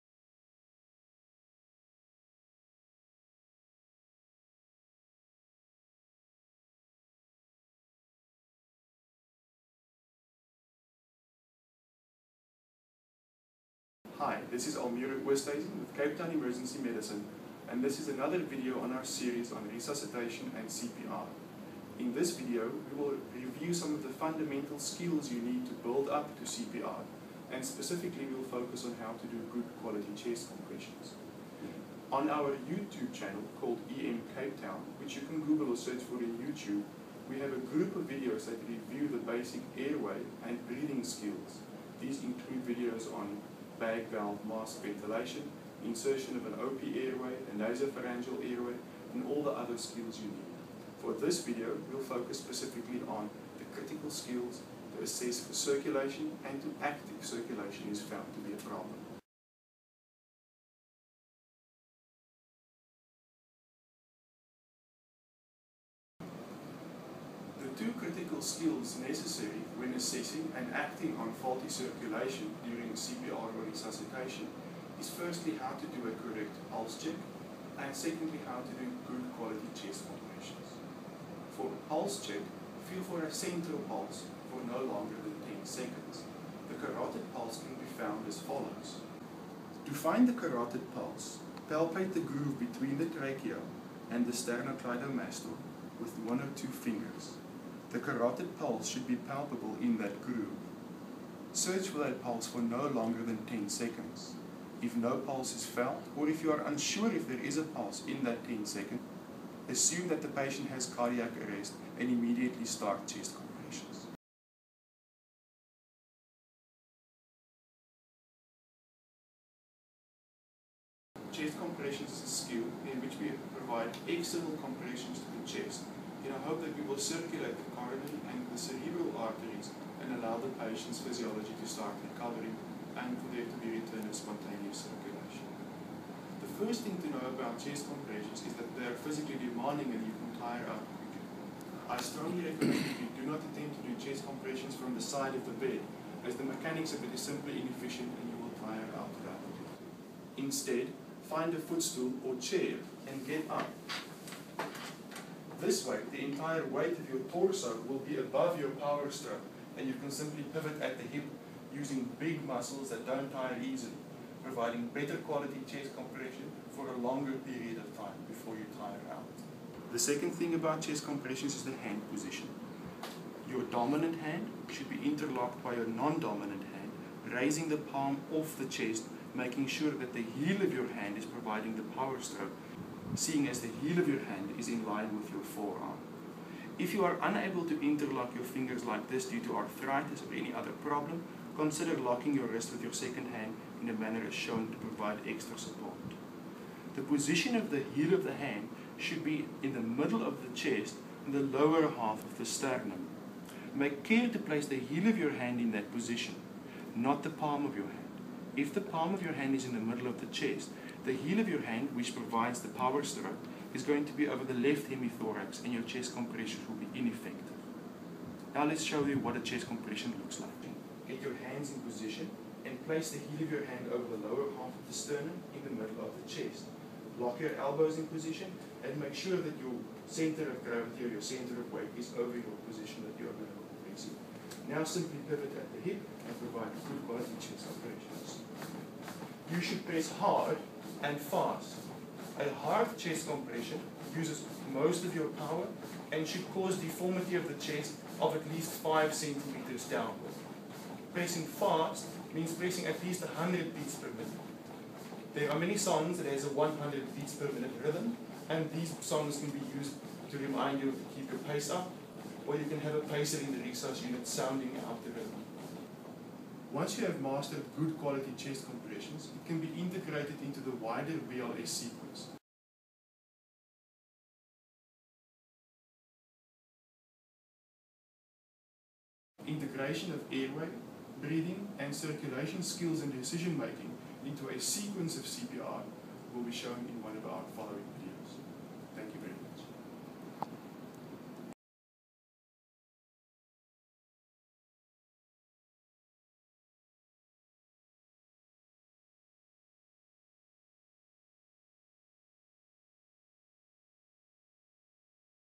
BLS Skills: Chest Compressions
How to perform pulse check and chest compressions by Cape Town Emergency Medicine